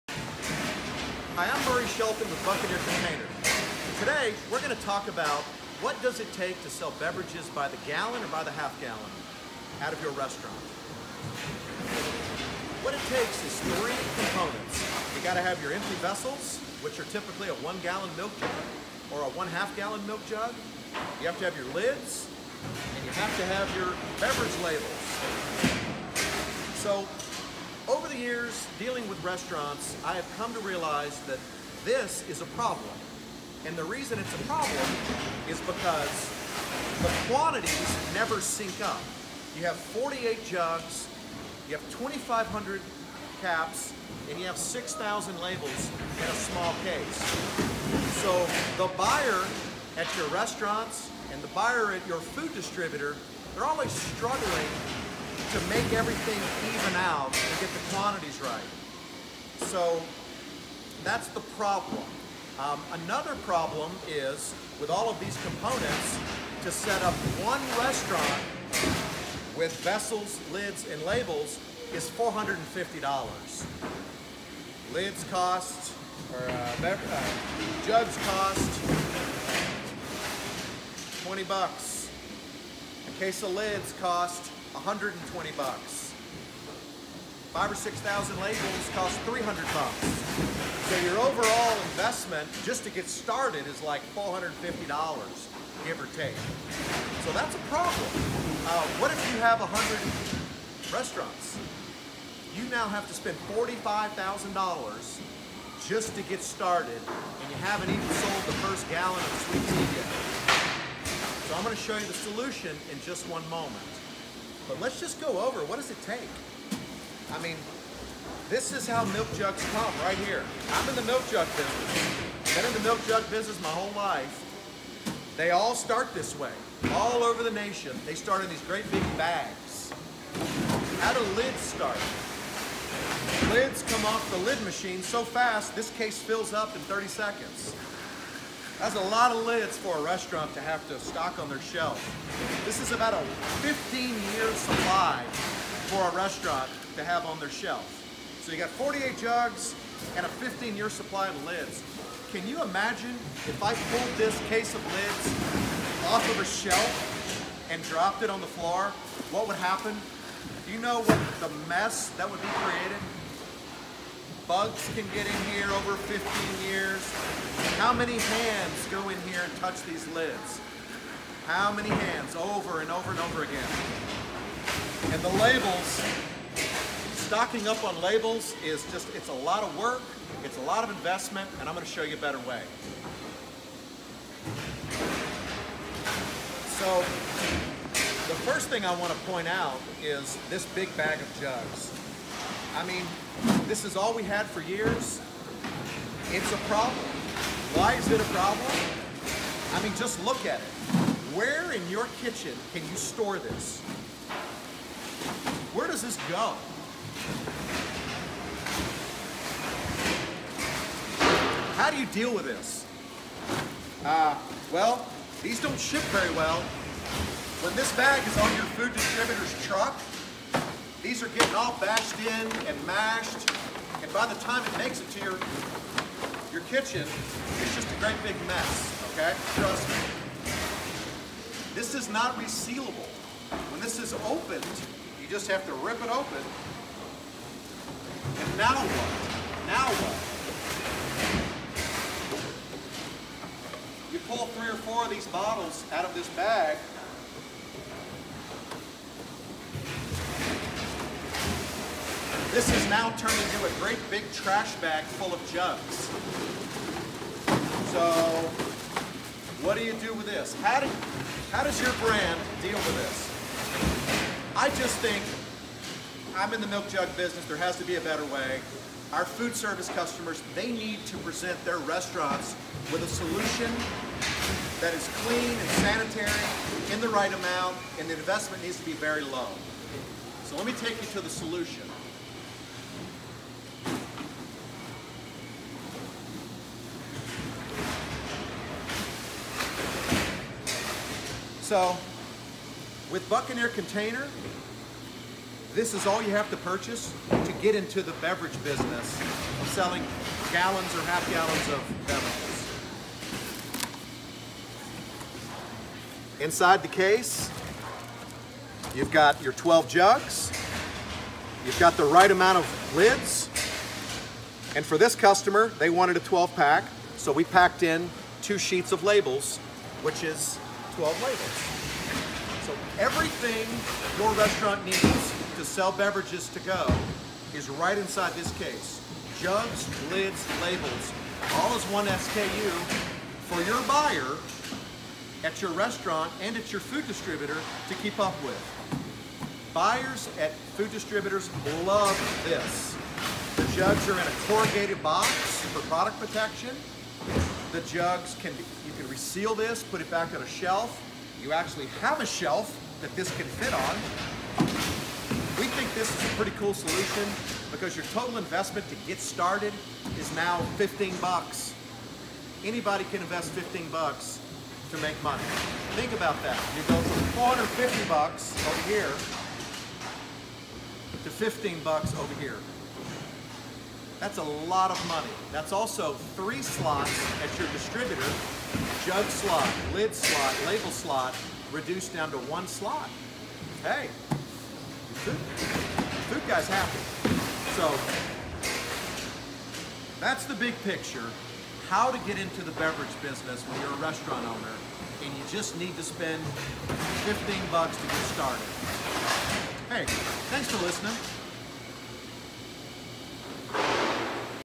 The PROBLEM & The SOLUTION - Get Started Selling Beverages By The Gallon Or 1/2 Gallon For Under $20
For Under $20 You Can Source The Beverage To-Go Packaging, And Your In The Bulk Beverage Business!! Make Huge Margins Selling Your Beverages To Your Existing To-Go, Delivery, and Catering Customers.